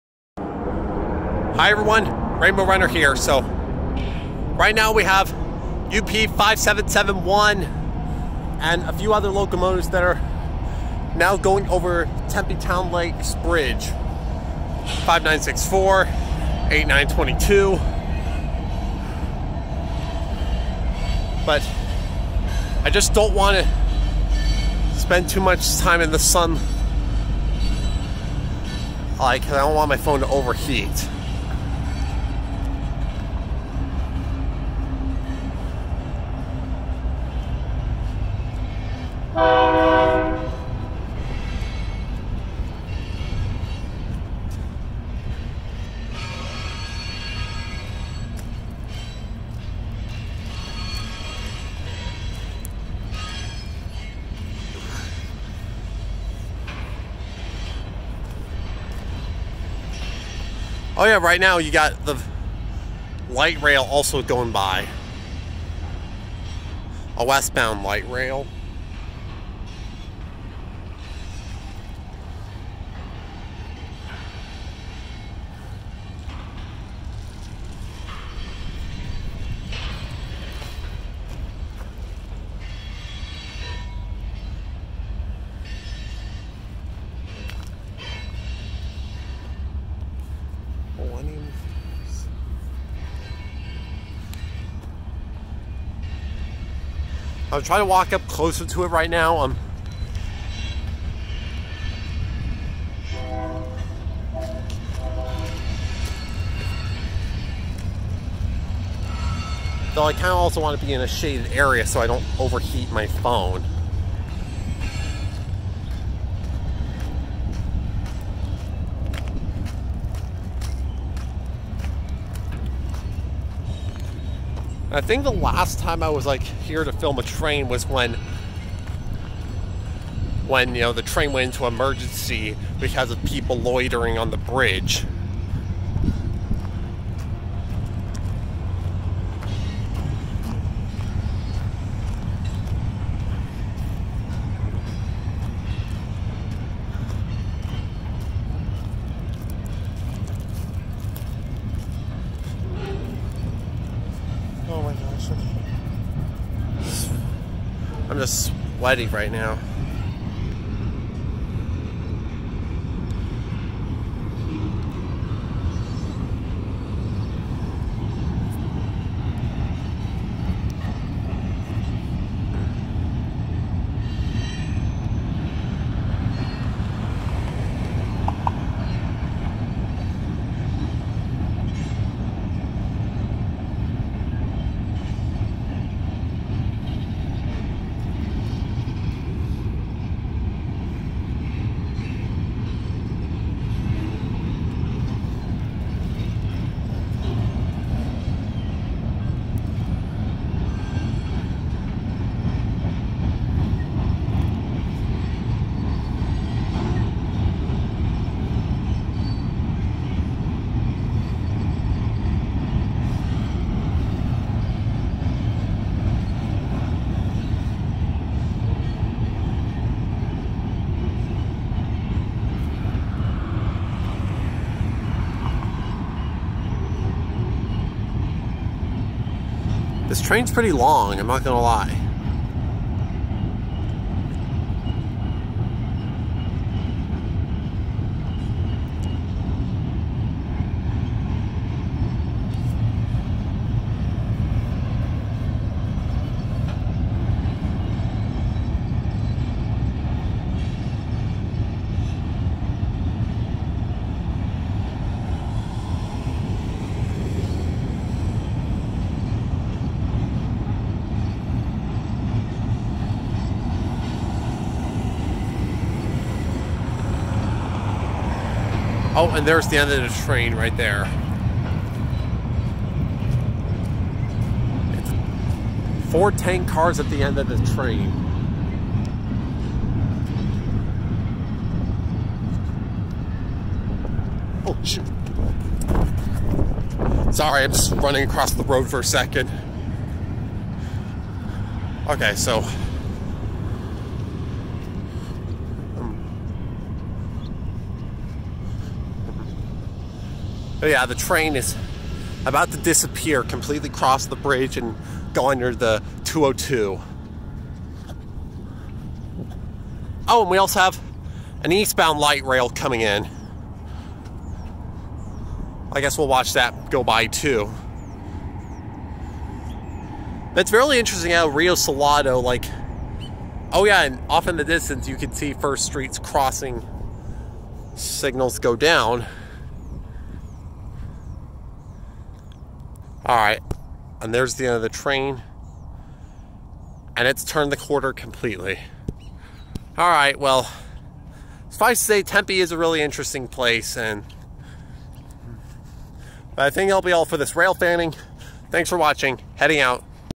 Railfanning #50: UP 5771 Leads Manifest Train Over Tempe Town Lake Bridge
(5/7/2020) Another train heading over Tempe Town Lake. UP 5771, 5964, and 8922 all leading the train fairly slowly. I was kinda walking while filming too.